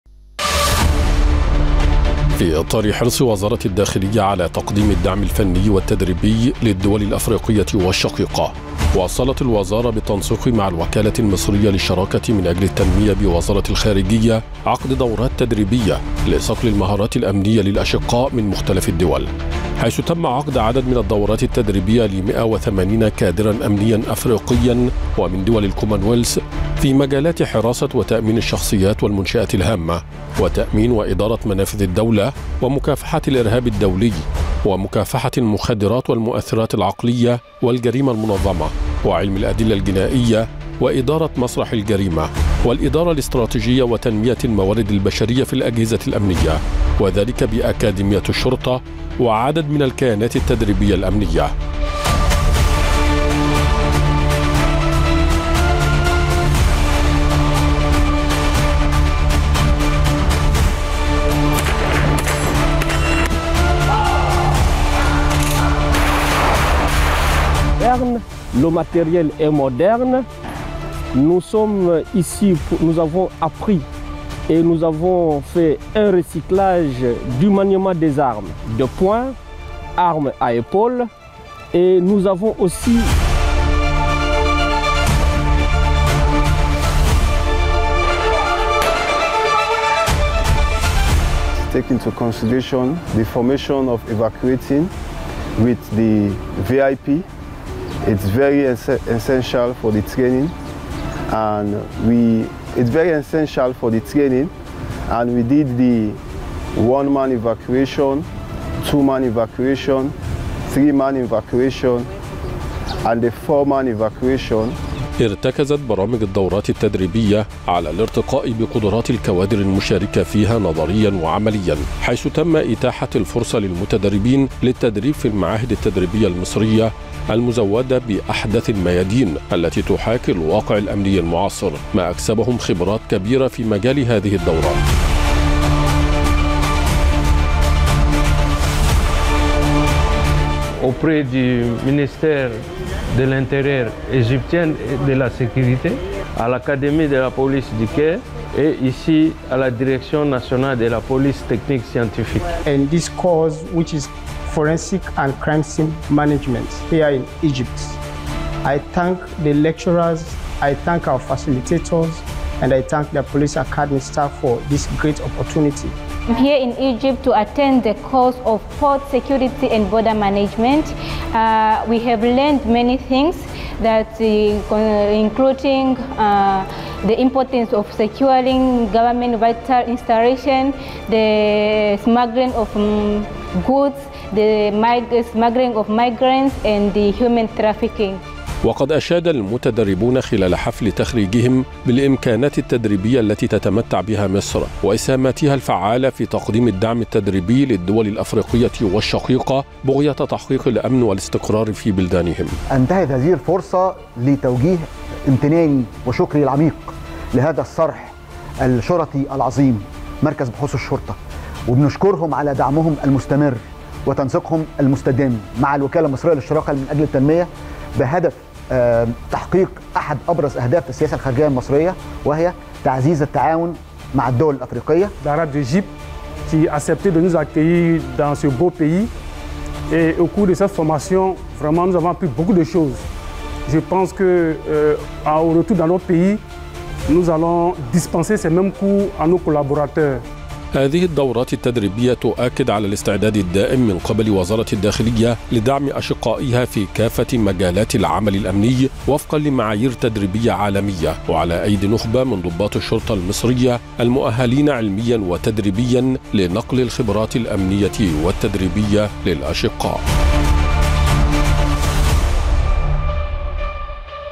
وزارة الداخلية تنظم عددا من الدورات التدريبية لعدد من الكوادر الأمنية من الدول الأفريقية ودول الكو
وزارة الداخلية تنظم عددا من الدورات التدريبية لعدد من الكوادر الأمنية من الدول الأفريقية ودول الكومنولث